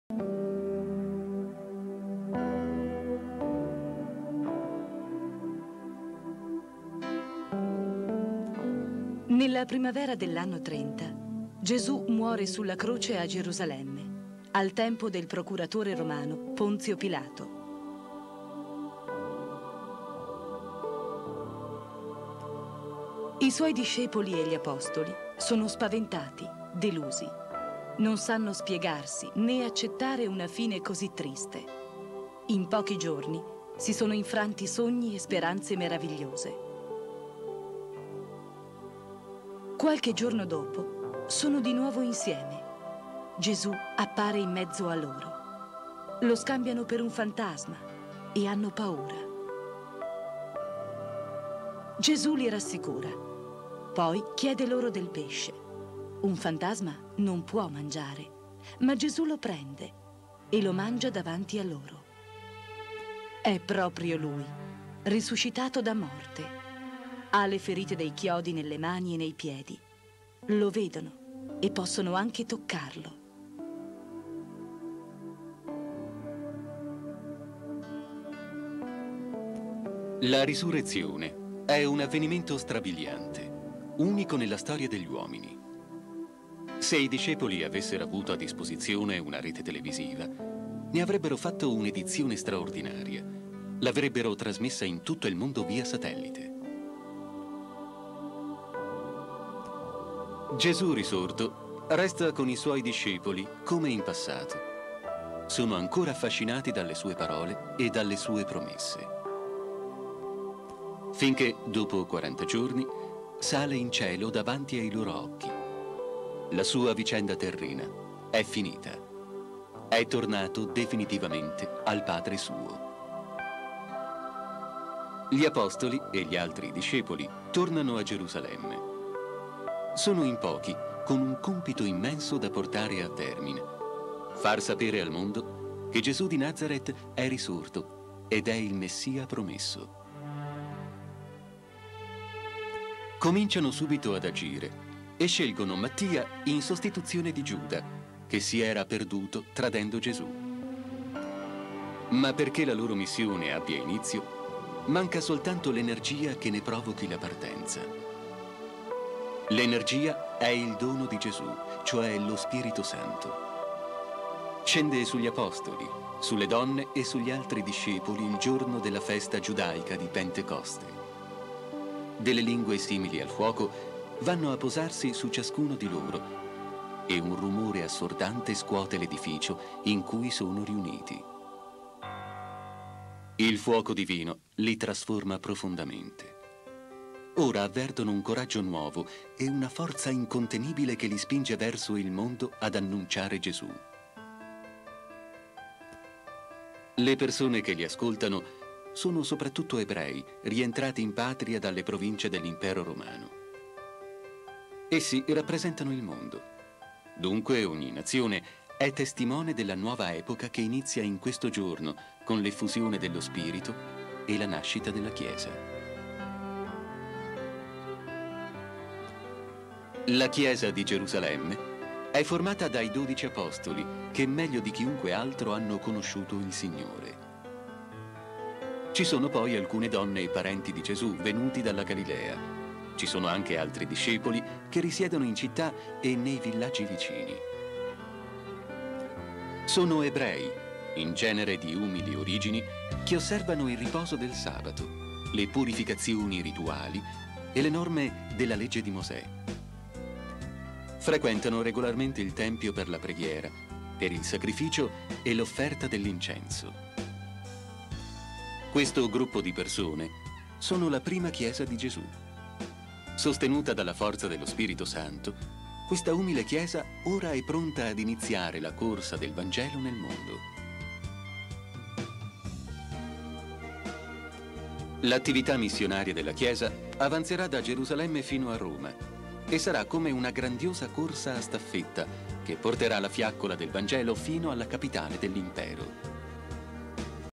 LA NASCITA DELLA CHIESA fumetti 1° parte
catechismo per ragazzi... la nascita della chiesa come ci viene raccontata dai vangeli e soprattutto dagli atti degli apostoli